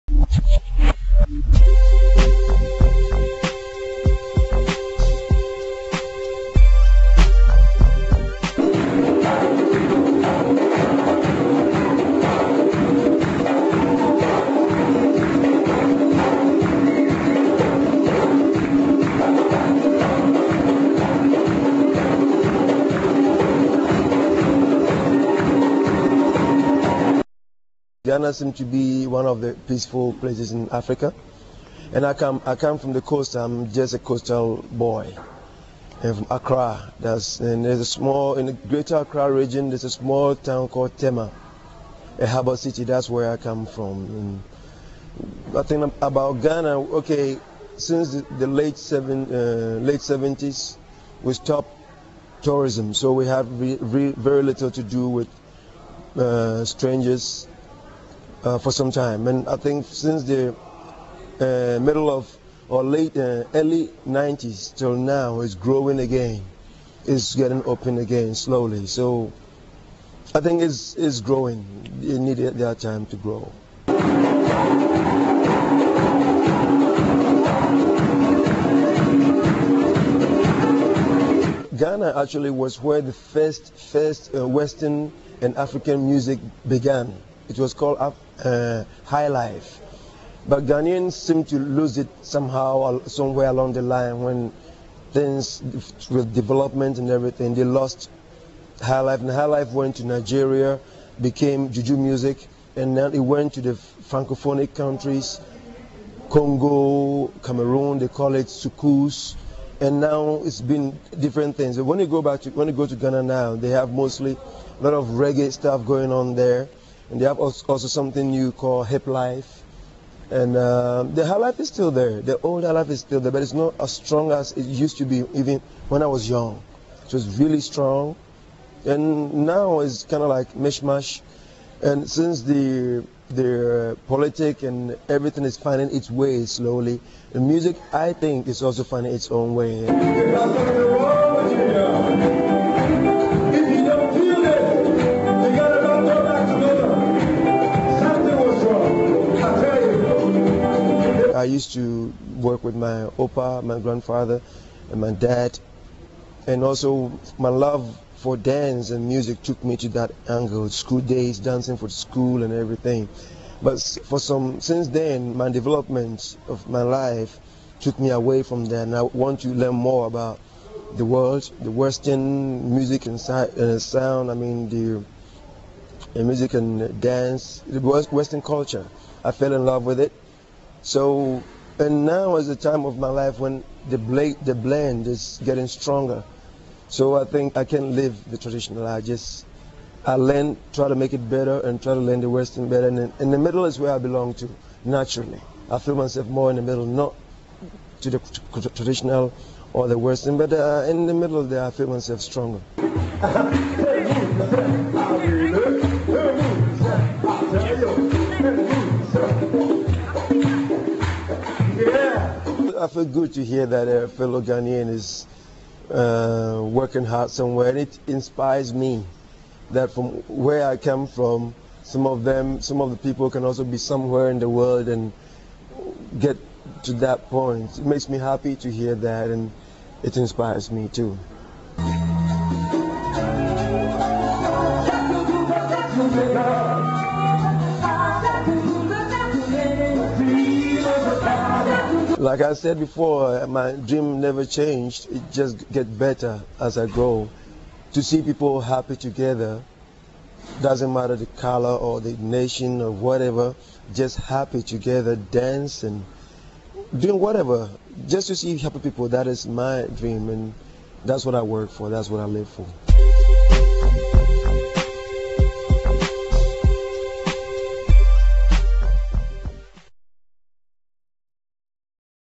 Singer and dancer Oboja Adu from Ghana show and interview part 2 @ web62.com Internet TV
Singer and dancer Oboja Adu from Ghana in an interview part 2 @ web62.com Internet TV looking at Ghana music.